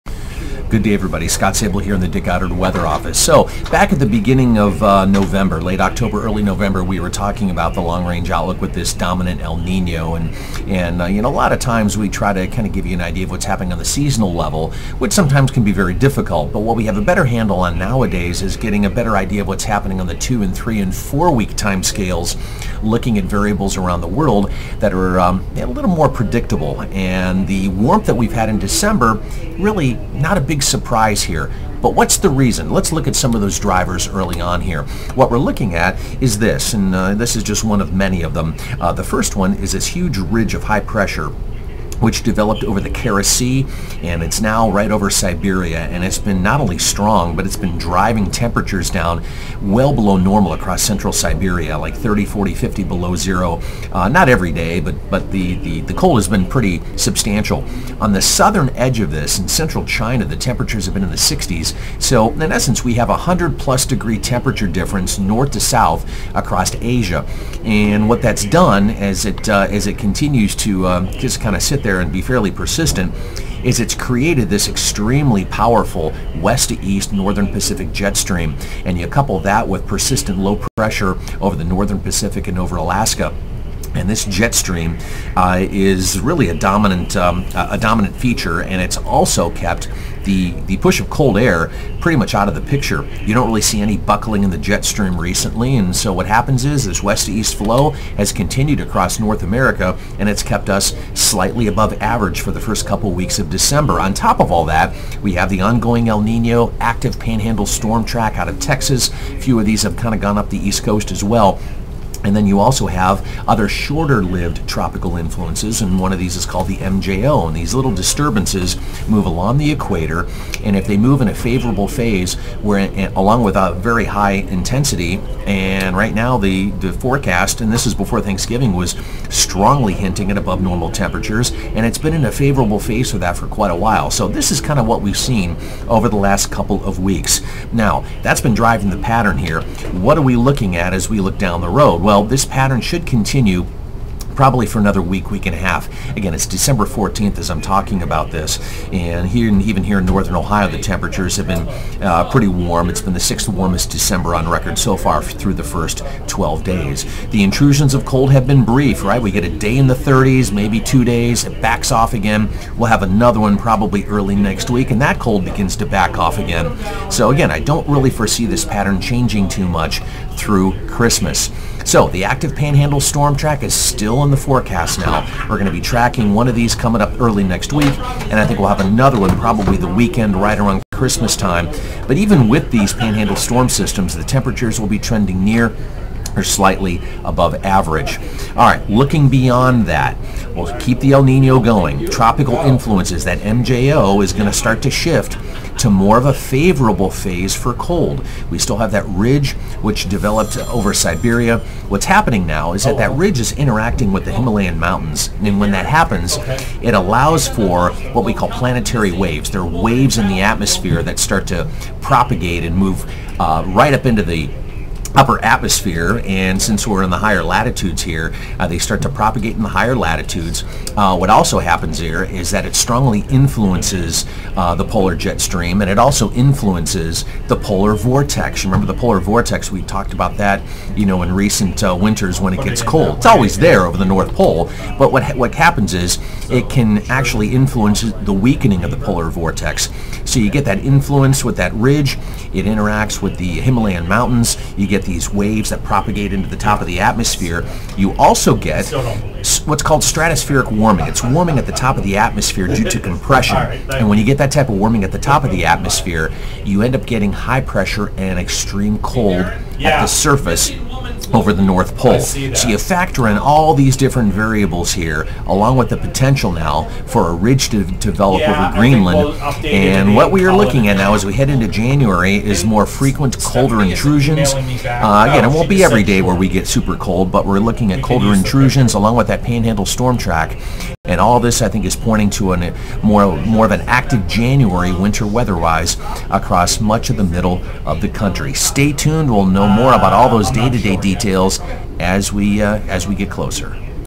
Why the Milder December 2023
A detailed explanation of the drivers behind the milder December (through mid month).  Second half is a look at the drivers behind the upcoming colder January.  I try to keep the explanation in layman's terms without too much technical weather.